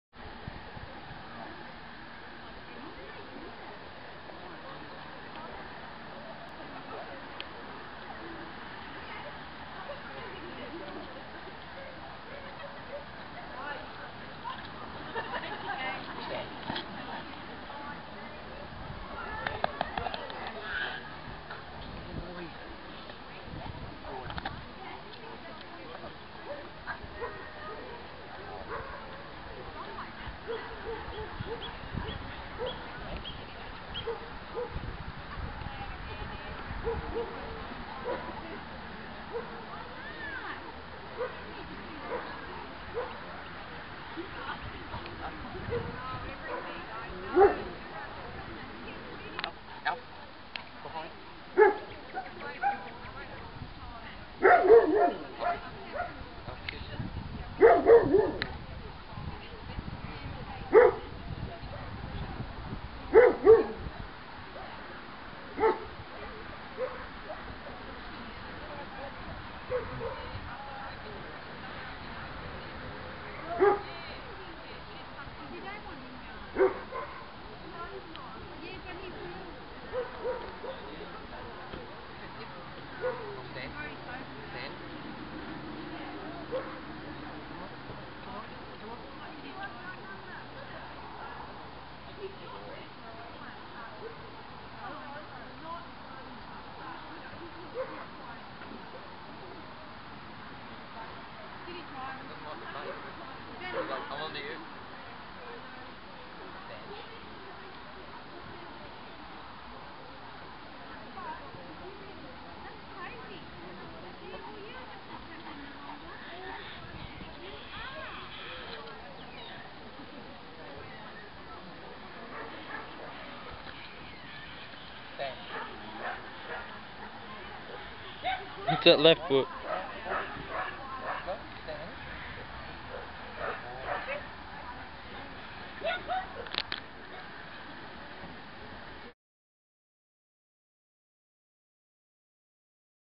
Alf 17th July
Bit of show training for Alf at 17mths old - 1st and 2nd attempts at free stacking.